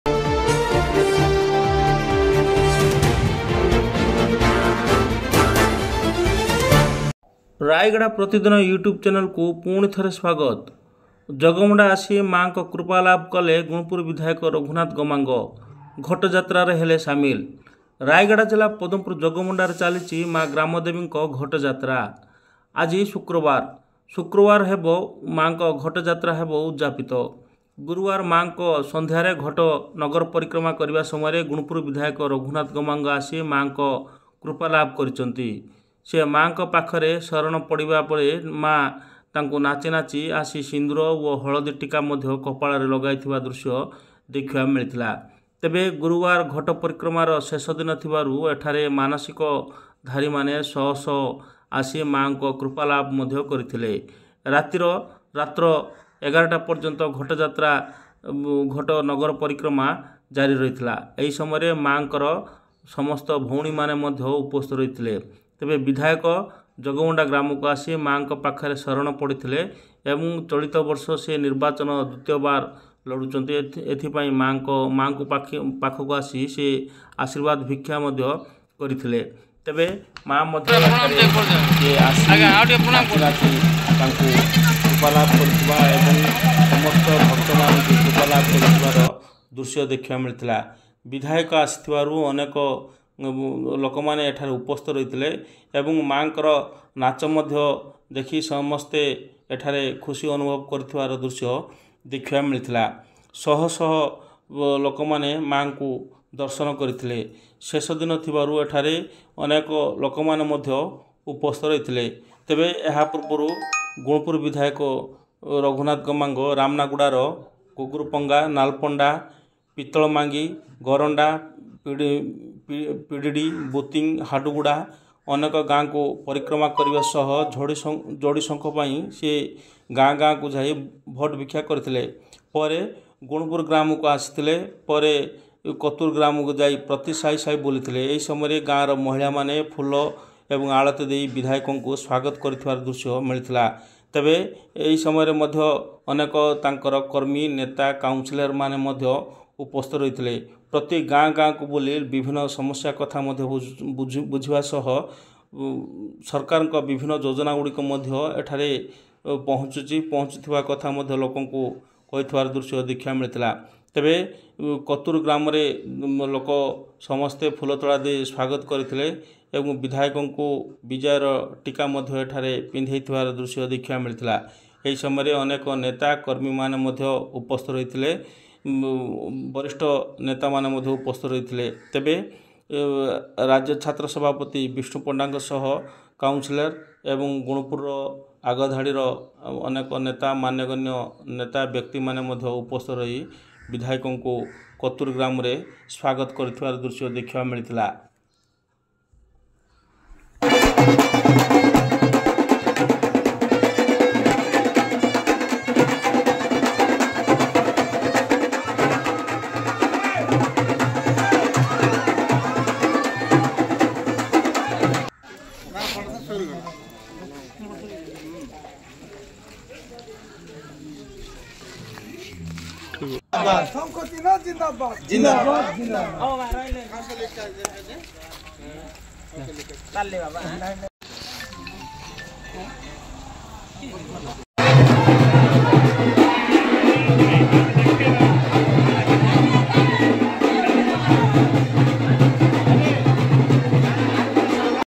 ଜଗମଣ୍ଡା ଗ୍ରାମକୁଆସି ମାଁ ଗ୍ରାମଦେବୀଙ୍କ ଘଟଯାତ୍ରାରେ ସାମିଲହେବା ସହ କୃପା ଲାଭକଲେ ଗୁଣପୁରବିଧାୟକ ରଘୁନାଥ ଗମାଙ୍ଗ
ମାଁ ଗ୍ରାମଦେବୀଙ୍କ ଶରଣ ନେଲେ ଗୁଣପୁର ବିଧାୟକ ରଘୁନାଥ ଗମାଙ୍ଗ

 ଜଗମଣ୍ଡା ଆସି ମାଁ ଙ୍କ କୃପା ଲାଭ କଲେ ଗୁଣପୁର ବିଧାୟକ ରଘୁନାଥ ଗମାଙ୍ଗ

ରାୟଗଡ଼ା ଜିଲ୍ଲା ପଦ୍ମପୁର ଜଗମାଣ୍ଡାରେ ଚାଲିଛି।ମା ଗ୍ରାମ ଦେବୀ ଙ୍କ ଘଟଯାତ୍ରା ଆଜି ଶୁକ୍ରବାର ହେବ ଉଡଯଦପିତ।ଗୁରୁବାର ମାଁ ଙ୍କ  ଘଟ ନଗର ପରିକ୍ରମା କରିବା ସମୟ ରେ ଗୁଣପୁର ବିଧାୟକ ରଘୁନାଥ ଗମାଙ୍ଗ ଆସି ମା ଙ୍କ କୃପା ଲାଭ କରିଛନ୍ତି।ସେ ମା ଙ୍କ ପାଖରେ ଶରଣ ପଡ଼ିବା ପରେ ତାଙ୍କୁ ମା ନାଚି ନାଚି ଆସୁ  ସିନ୍ଦୂର ସହ ହଳଦୀ ଟିକା କପାଳରେ ଲଗାଇ ଥିଲେ ।ତେବେ ଗୁରୁବାର ଘାଟ ପରିକ୍ରମାର ଶେଷ ଦିନ ଥିବାରୁ ଶହ ଶହ ମାନସିକ ଧାରୀ ମାନେ ଆସି ମାଁ ଙ୍କ ସହ ପରିକ୍ରମା କରିଥିଲେ।ଏହି ସମୟ ରେ ନେତା ଗୋଦାବରୀ ରାଉତ,ଦୁର୍ଗା ସାହୁ ଅନେକ ବିଜେଡି କର୍ମୀ ମାନେ ଉପସ୍ଥିତ ରହିଥିଲେ।ଏହା ପୂର୍ବରୁ ବିଧାୟକ ରଘୁ ନାଥ ଗମାଙ୍ଗ ରାମାନା ଗୁଡ଼ା ର Gugurpanga, ନାଲ୍ପଣ୍ଡଗୁଡ଼ା, ପିତଳମାଙ୍ଗି, ଗରଣ୍ଡା, ପଡ଼ିଡି, ବୁଥିଂ, ହାଡୁଗୁଡ଼ା, ଅନେକ ଗାଁ ପରିକ୍ରାମ୍ କରିବା ସହ ଯୋଡ଼ି ଶଙ୍ଖ ପାଇଁ ଭୋଟ ଭିକ୍ଷା କରିଥିଲେ।ପରେ ସେ ଗୁଣପୁର କତ୍ତୁରୁ ଗ୍ରାମ କୁ ଯାଇ ପ୍ରତି ସାହି ସାହି ବୁଲି ଥିଲେ।ଗାଁ ର  ମହିଳା ମାନେ ଫୁଲ,ଆଳତି କରିଁ ବିଧାୟକ ଙ୍କୁ ସ୍ଵାଗତ କରିଥିଲେ।ଏହି ସମୟ ରେ ଭାଷା ଶତପଥୀ,ରାମ ପଣ୍ଡା ରାଜ୍ୟ ଛାତ୍ର ସଭାପତି,ବିଷ୍ଣୁ ପଣ୍ଡା  ଙ୍କ ସହ କାଉ ନ ସିଲର ଅନେକ ବିଜେଡି କର୍ମୀ ମାନେ ଉପସ୍ଥିତ ରହିଥିଲେ।